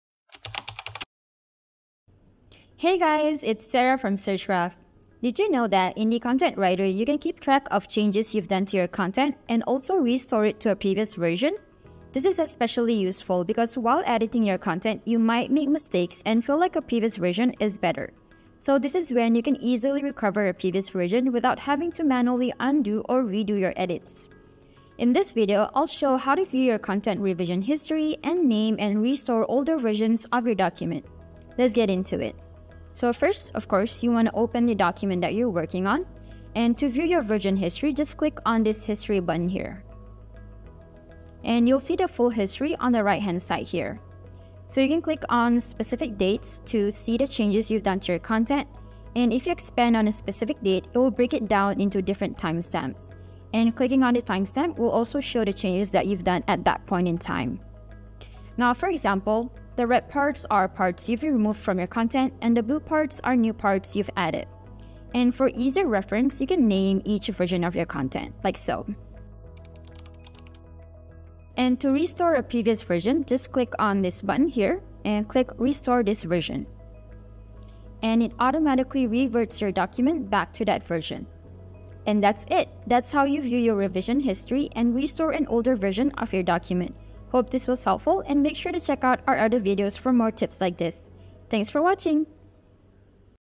How to View Revision History & Restore Previous Versions
Accidentally made too many edits to your content and wish to go back in time? Sure thing.

This video shows you how you can view your content revision history to keep track of changes you've made to your content, and how to restore an older version of your document.

⏳ Timestamps ⏳
0:00 Intro
0:33 How to view revision history
1:05 How to name versions
1:11 How to restore a previous version
1:21 Thanks for watching!